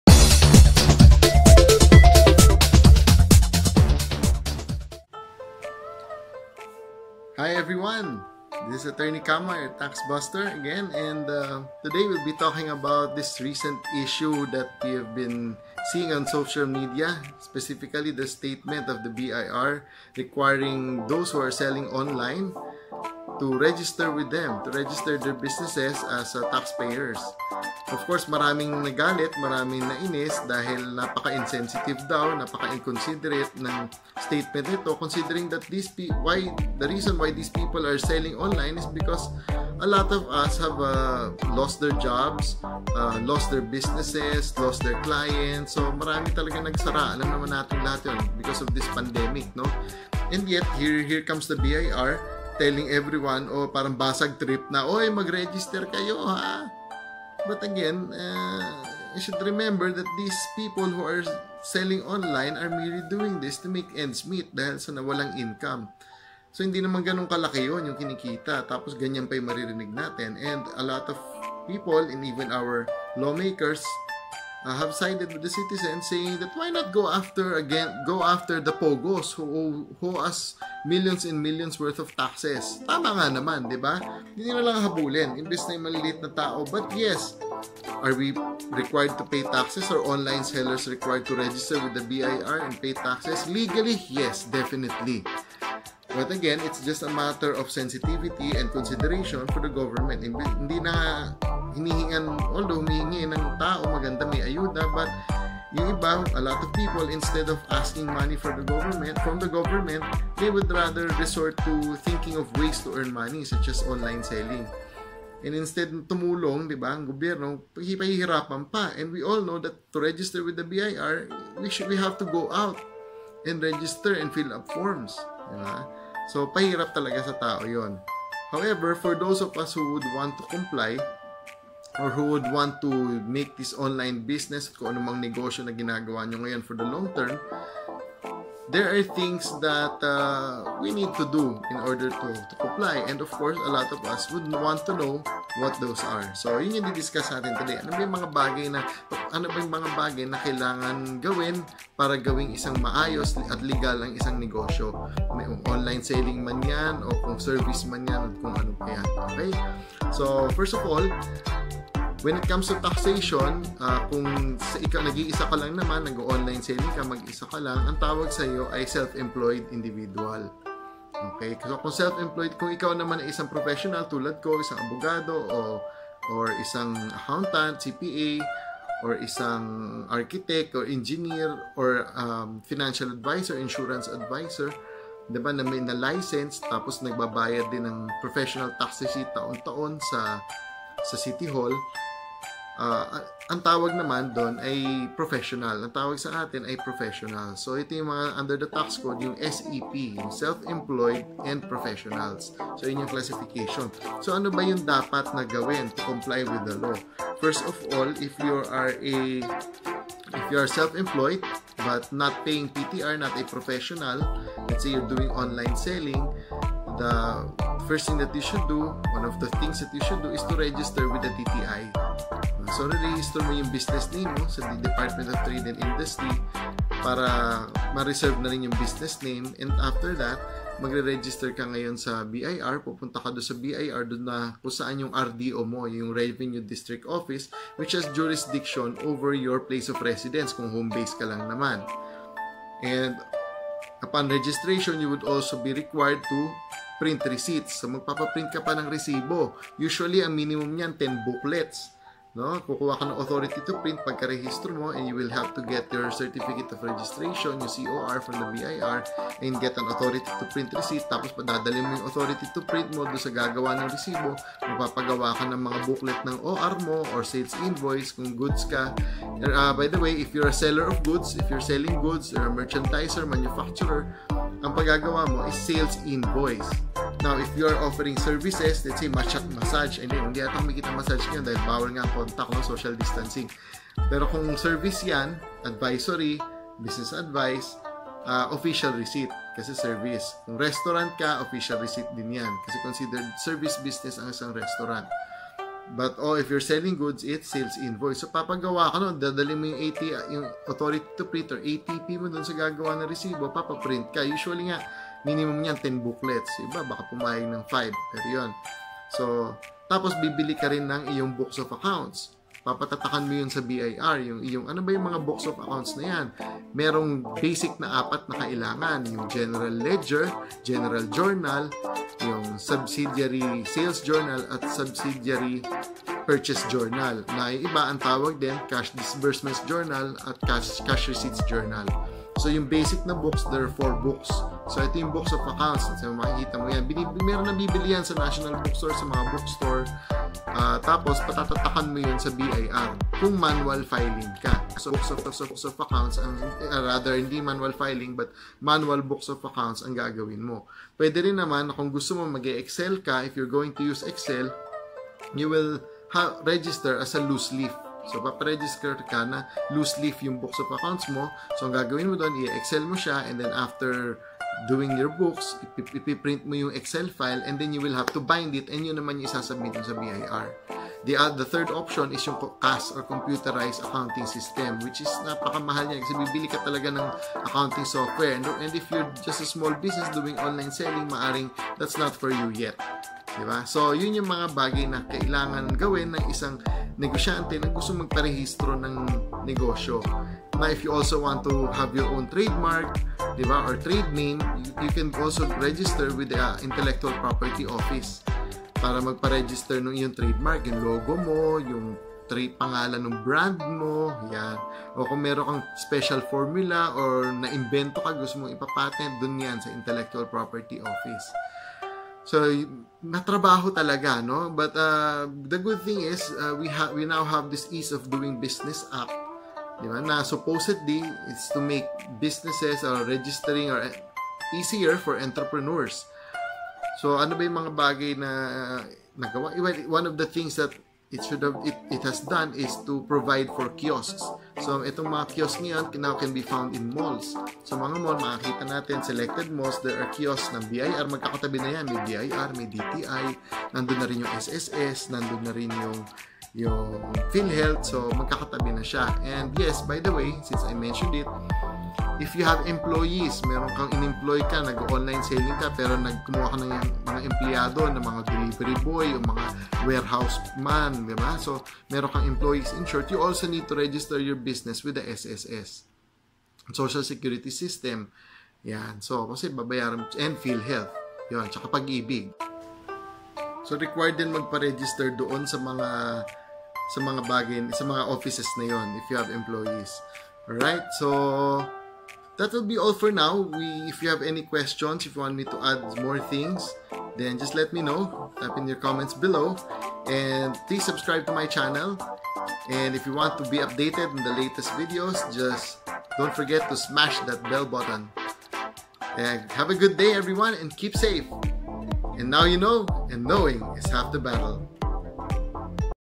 BIR Registration for Online Sellers?
The BIR recently mandated online sellers, including those selling during this COVID-19 pandemic in order to make ends meet.

In this video, we will discuss the issue.